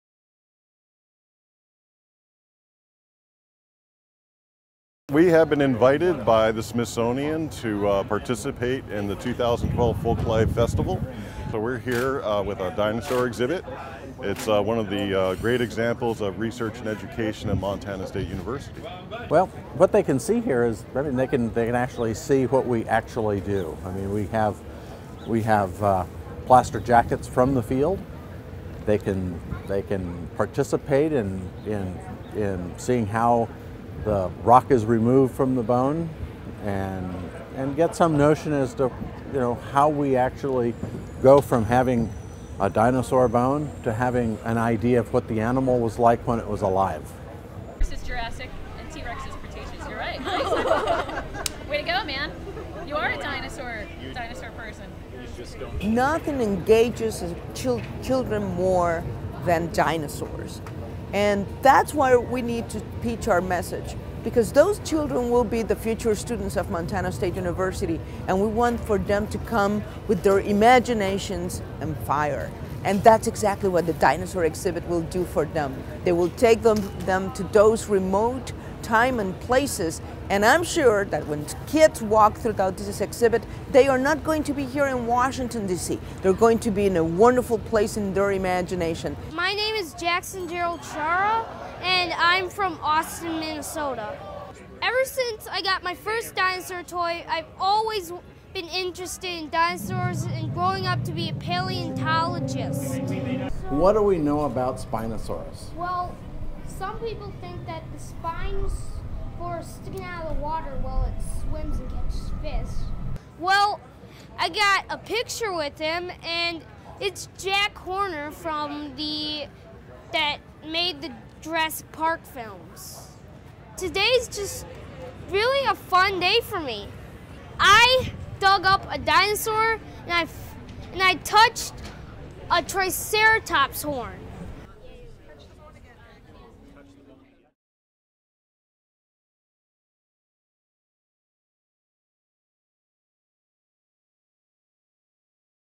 Montana dinosaurs at the Smithsonian Folklife Festival
The Museum of the Rockies at Montana State University has brought a paleontology exhibit to the Smithsonian Folklife Festival on the National Mall in Washington D.C. Visitors can learn what it's like to be a paleontologist and try their hand at digging for dinosaurs.